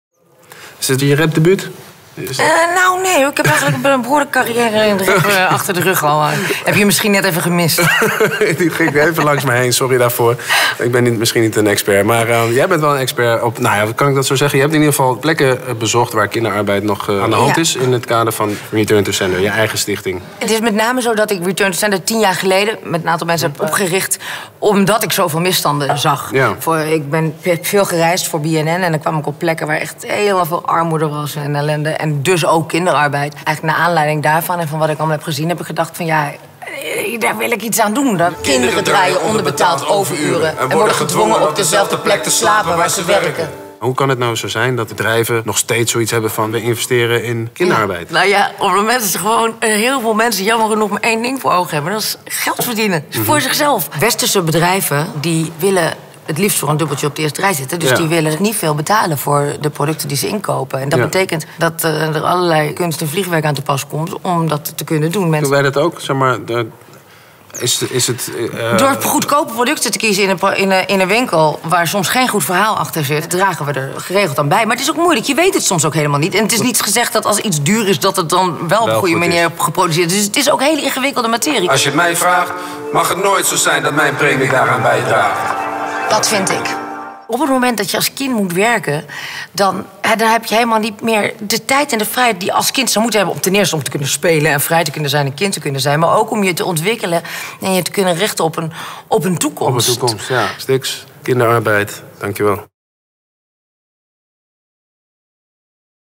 Sticks interviewt Katja Schuurman - het andere bewijs
Stichting Return to Sender maakt zich sterk voor duurzame producten, gefabriceerd zonder kinderhanden. Oprichtster Katja Schuurman zag met eigen ogen welke misstanden er zijn en vertelt hierover.